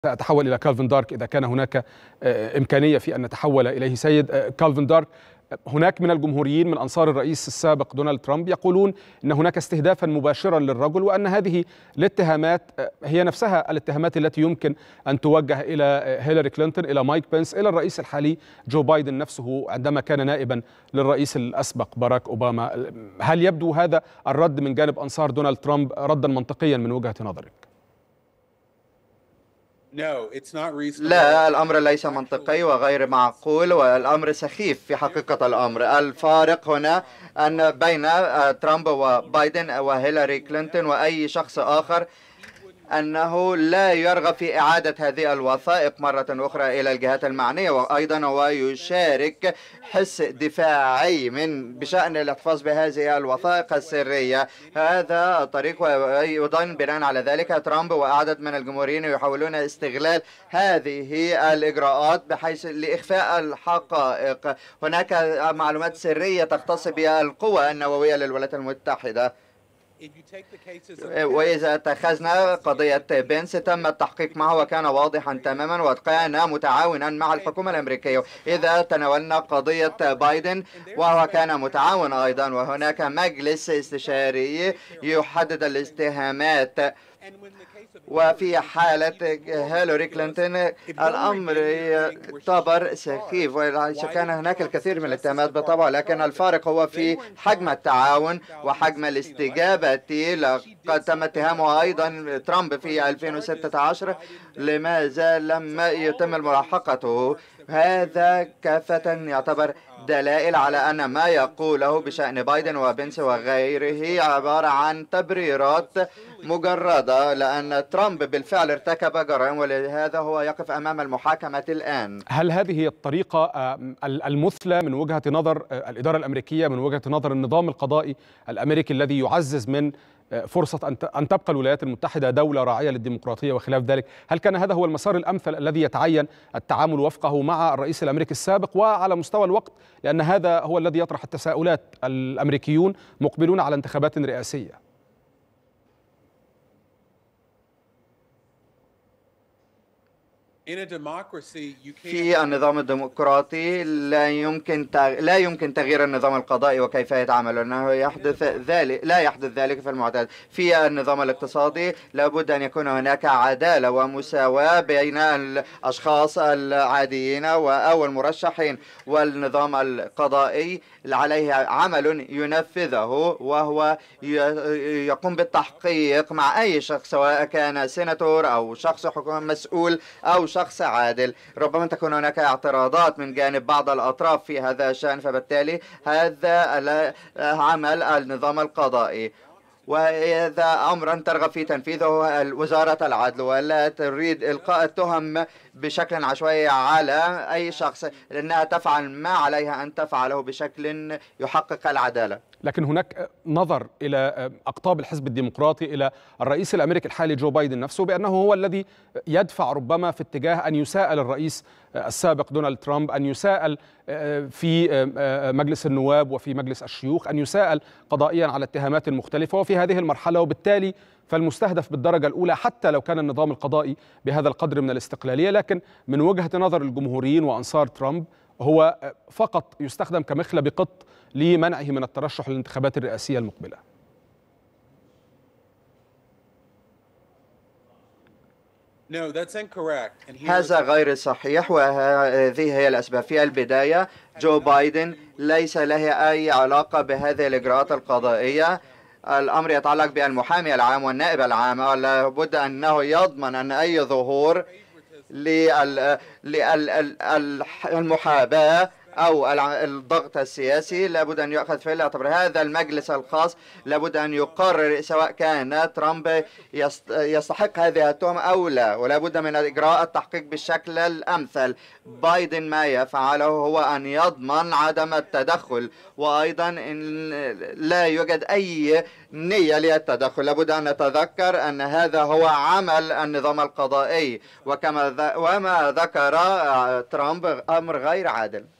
الانتخابات الأمريكية.. هل يسعى بايدن لسجن ترامب لمنعه من خوض السباق الرئاسي؟
الانتخابات الأمريكية.. هل يسعى بايدن لسجن ترامب لمنعه من خوض السباق الرئاسي؟|كالفين دارك خبير استراتيجي يوضح

13/6/2023